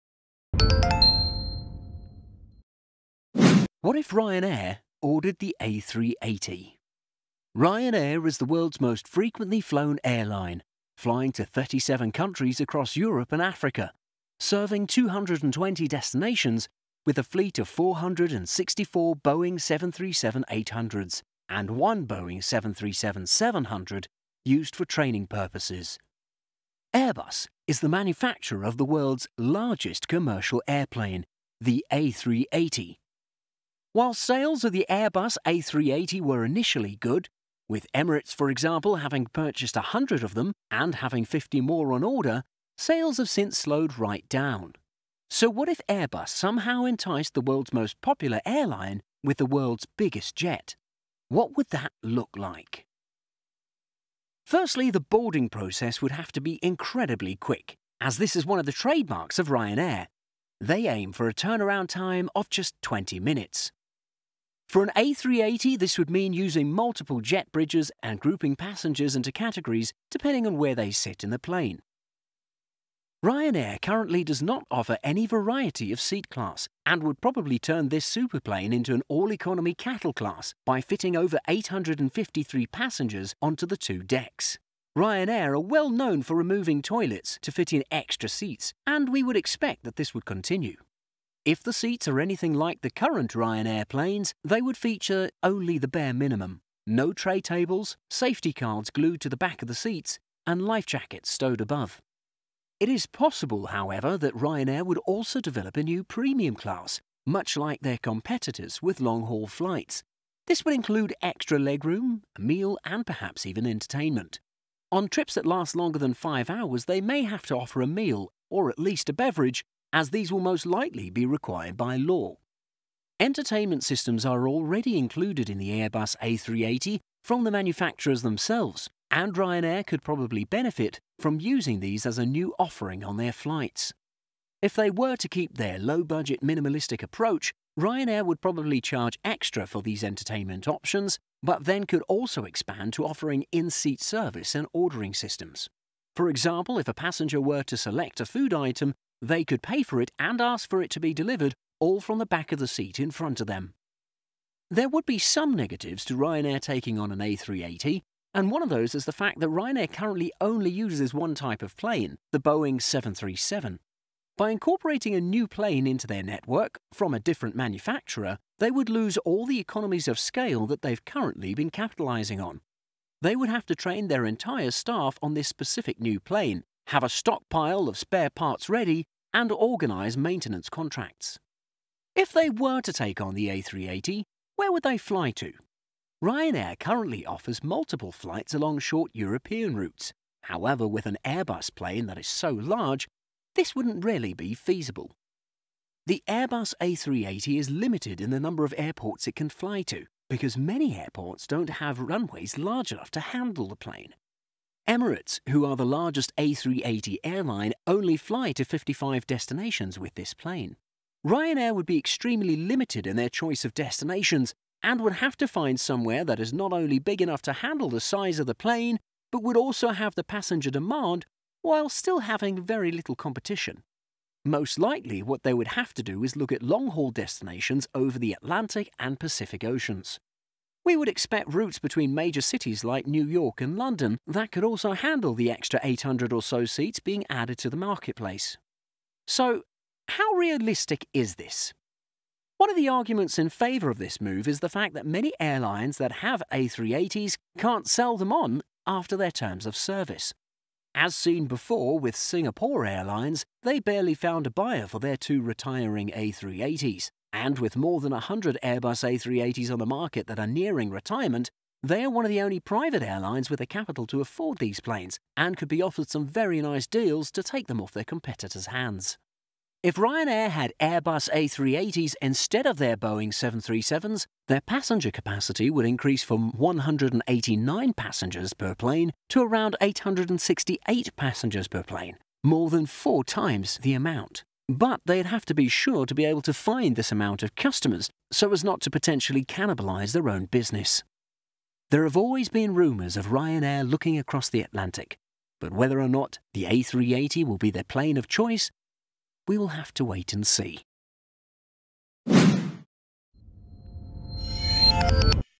What If Ryanair Orders A380's?
What if Ryanair Orders Airbus A380's?
- Where would they fly them?
- How would it affect the low cost flying industry?
- How likely is this to actually happen?